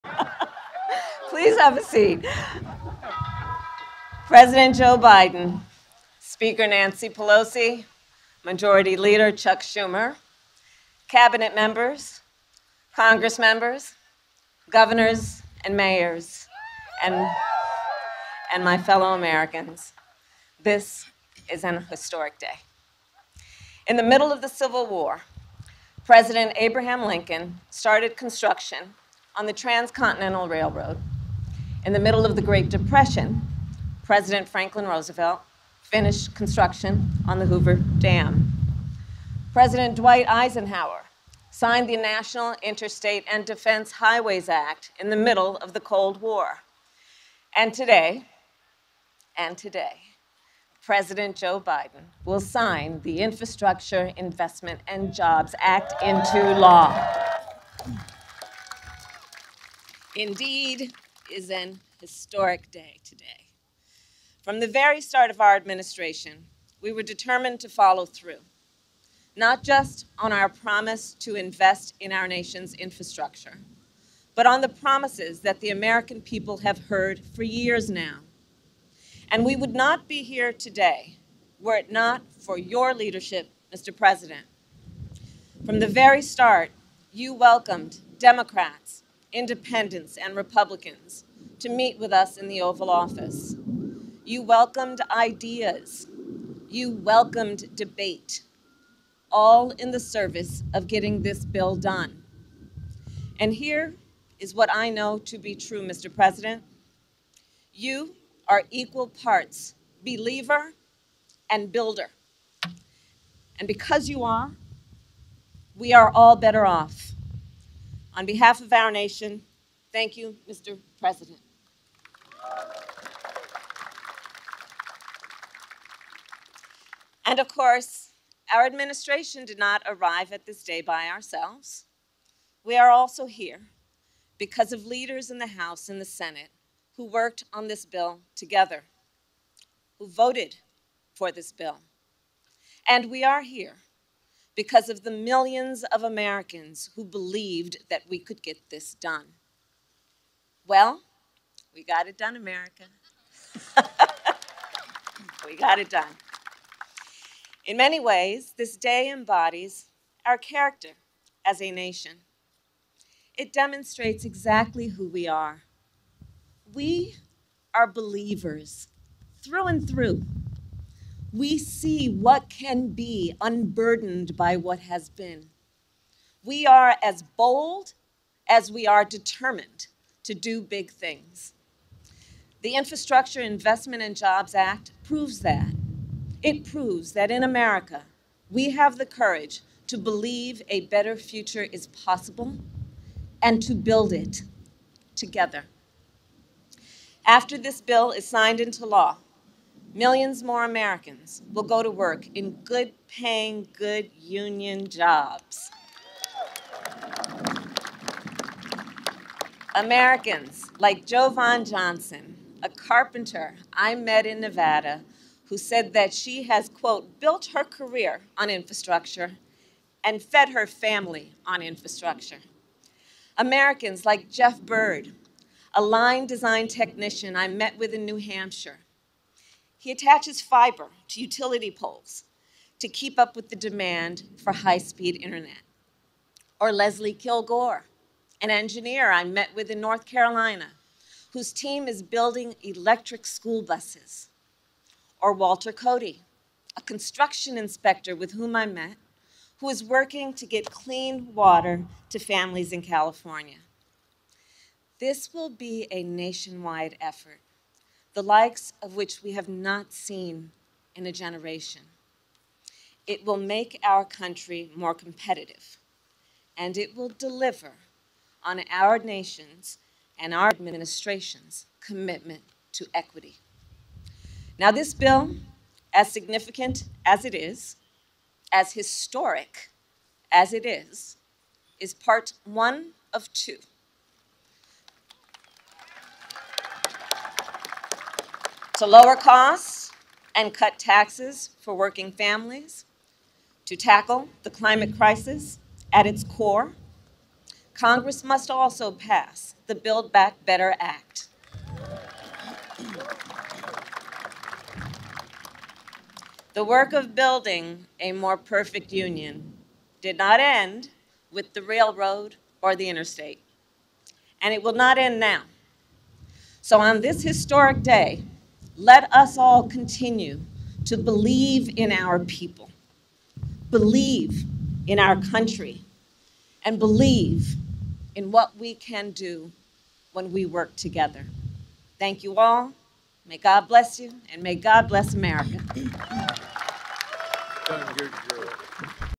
'We Got It Done, America': VP Kamala Harris Celebrates Passage Of Bipartisan Infrastructure Bill
At a White House event, Vice President Kamala Harris celebrated President Biden signing the bipartisan infrastructure bill into law.